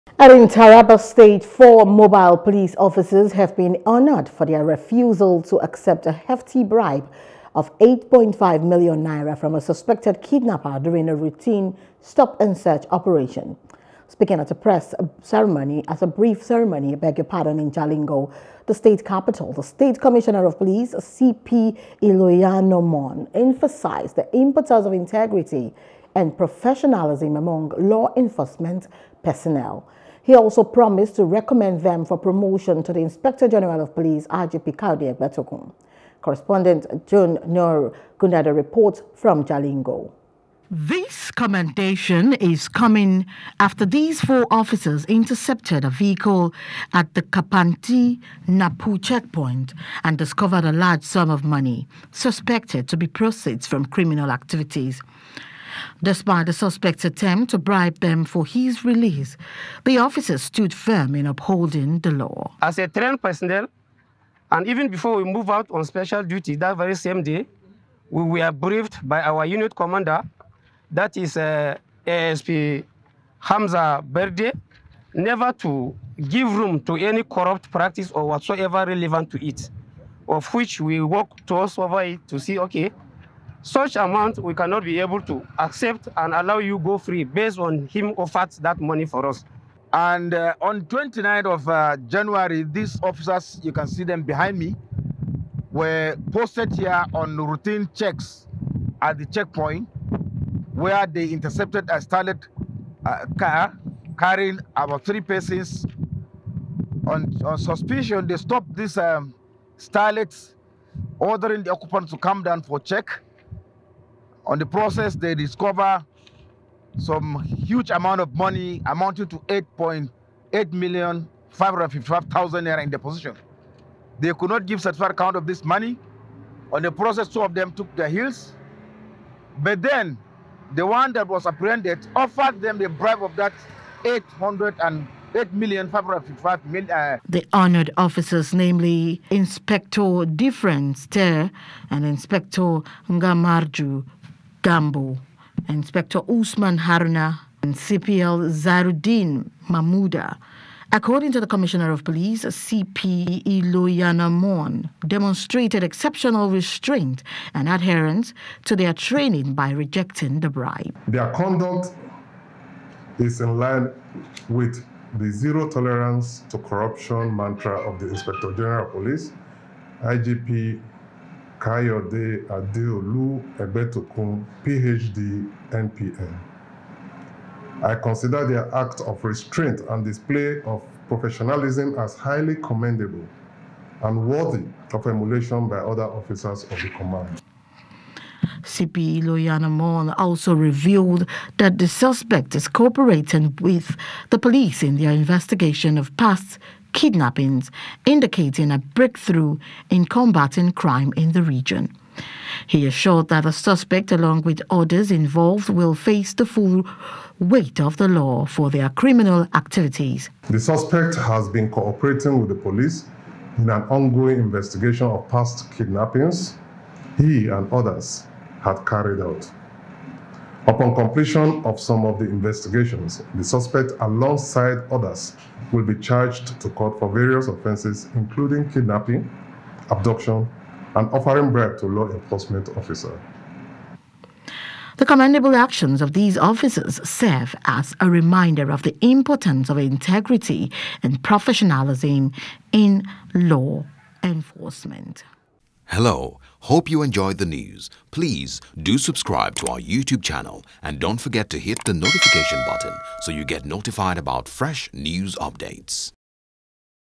Taraba Command Rewards Four Mobile Police Officers For Rejecting N8 5m Bribe
In Taraba State Four Mobile Police Officers have been honored for their refusal to accept a hefty bribe of N8.5 million from a suspected kidnapper during a routine stop-and-search operation.